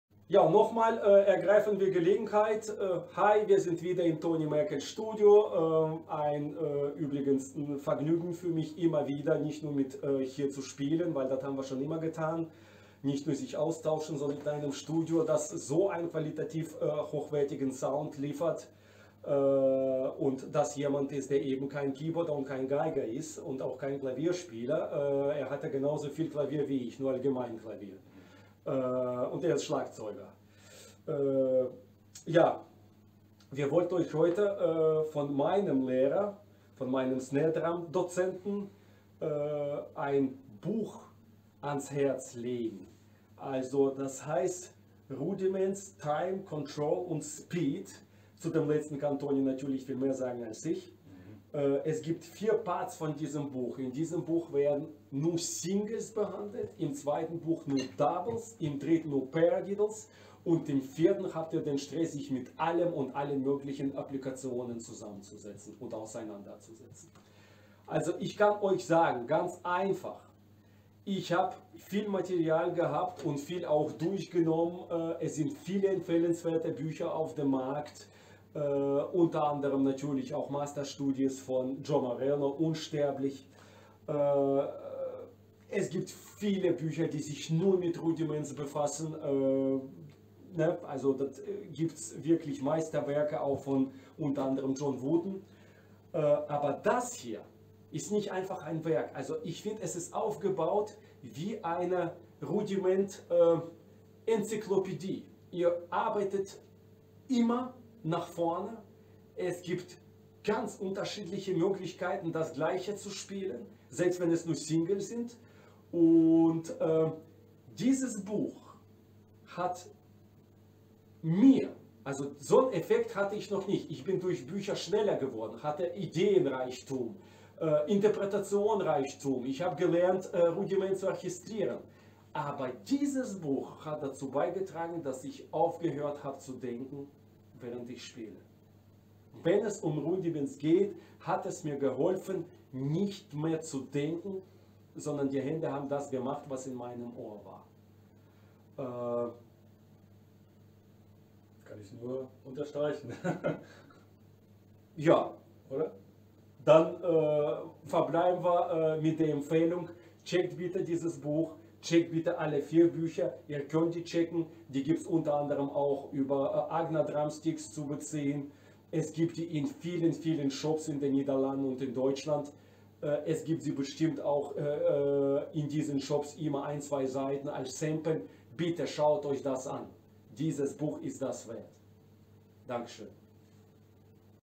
Yevgen Shestopalov and Toni Merkel about "Rudiments Time Control Speed"
Two great professionals, musicians of a number of legendary German bands about my books Rudiments Time Control Speed!